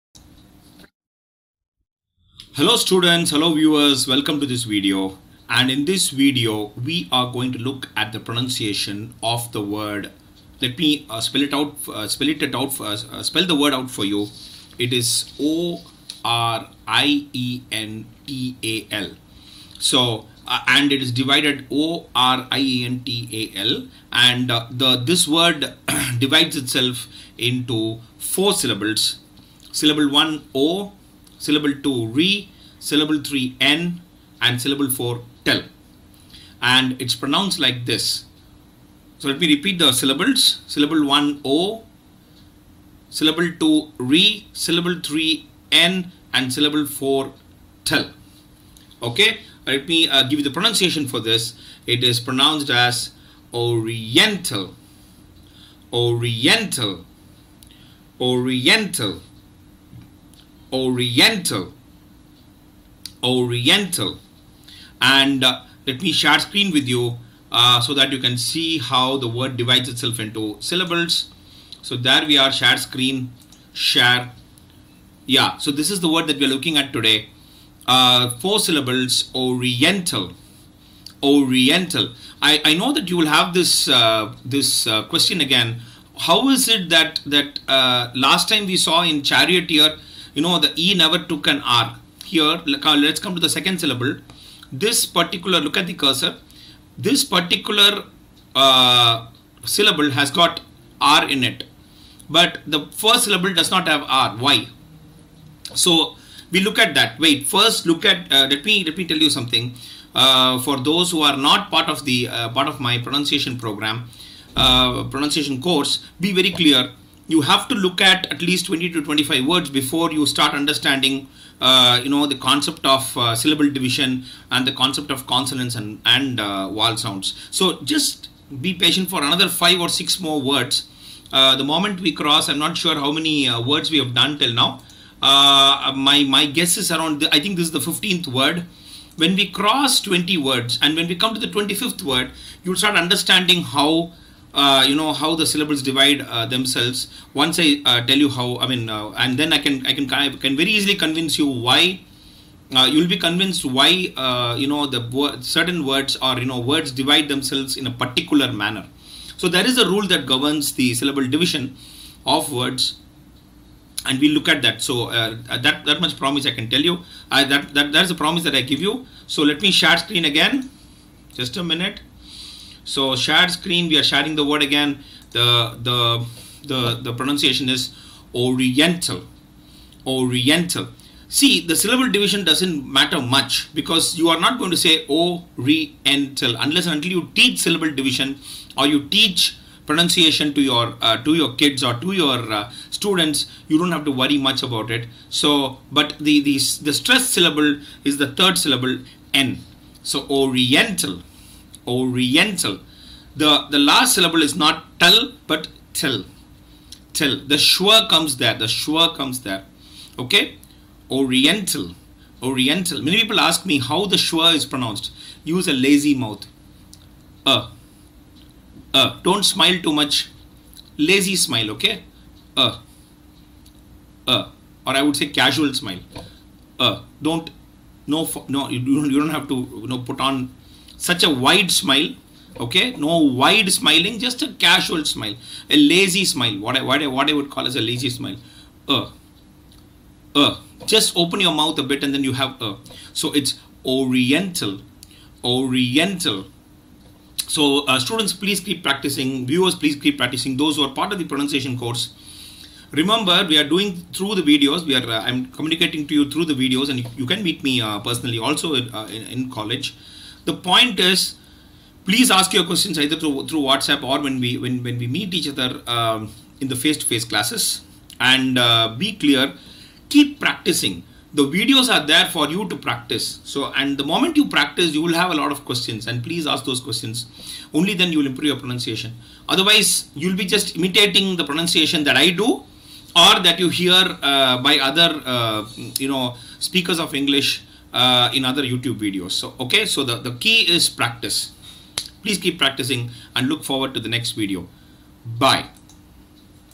Oriental Pronunciation Prof RBS
Dear Viewers, Dear Students. In this video, we are pronouncing the word "oriental". The word divides itself into four syllables and the third syllable is stressed.

Regards
Prof.R.Balasubramanian